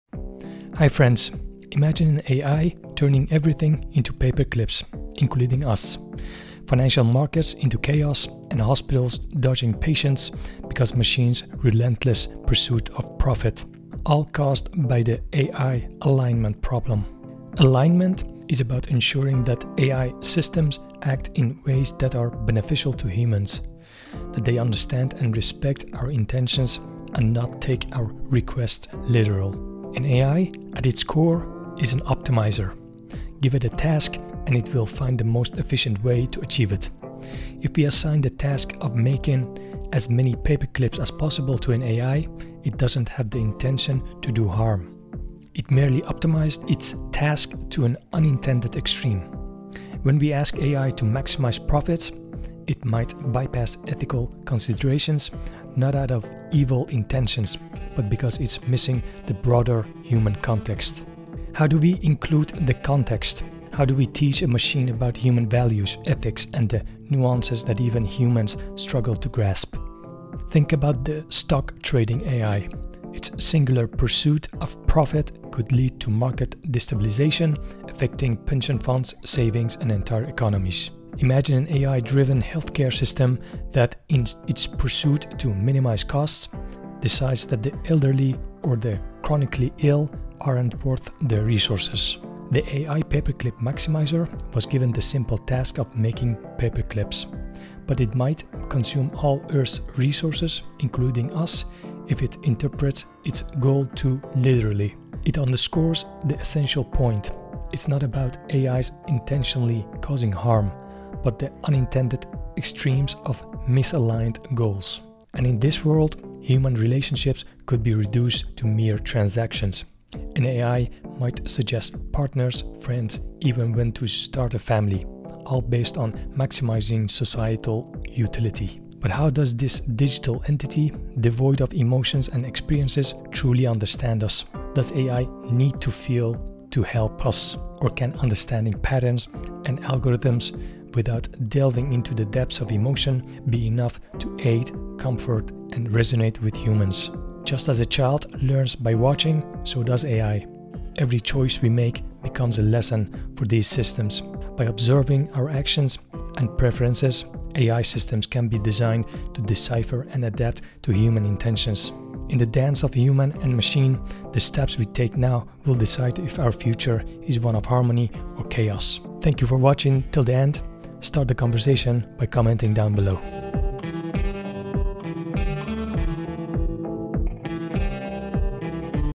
AI Alignment Problem: Extremes of Optimization
The AI alignment problem is the challenge of ensuring that artificial intelligence systems act ethically and in accordance with human intentions. It involves preventing unintended harmful outcomes caused by AI's optimization-focused decision-making. Solving this problem requires aligning AI's actions with human values while considering the complex nuances of ethics and human context.

Timestamp:
0:00 Intro
0:17 Alignment Problem
0:29 AI is an Optimizer
1:00 Giving AI Context
1:31 Humans into Paperclip
1:54 Human Relationships as Transactions
2:08 Understanding Through Algorithms